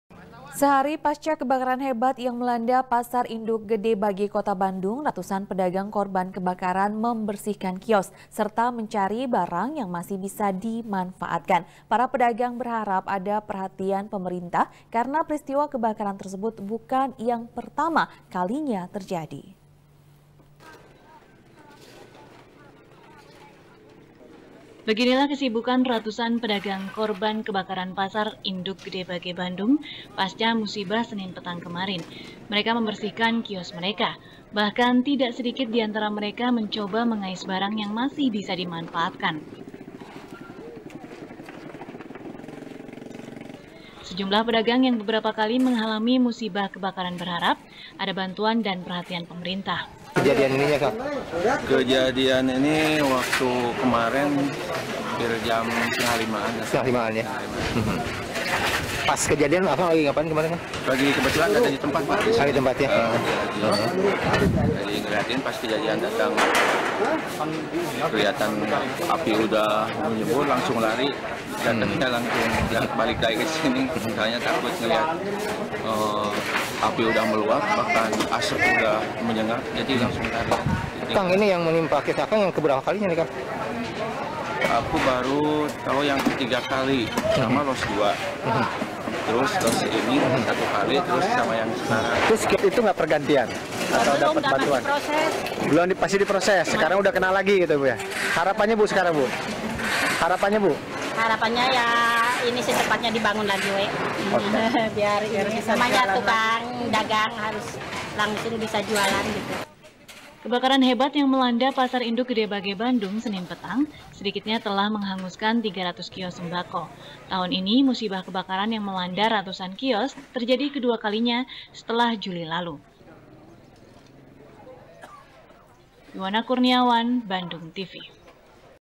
Pedagang Harapkan Bantuan Pasca Kebakaran | SBR | BANDUNG TV
SBR | BANDUNG TV | KRISANTI | Sehari pasca kebakaran hebat yang melanda pasar induk gede bage kota bandung, ratusan pedagang korban kebakaran membersihkan kios, serta mencari barang yang masih bisa dimanfaatkan. para pedagang berharap, ada perhatian pemerintah, karena peristiwa kebakaran tersebut bukan yang pertama kalinya terjadi.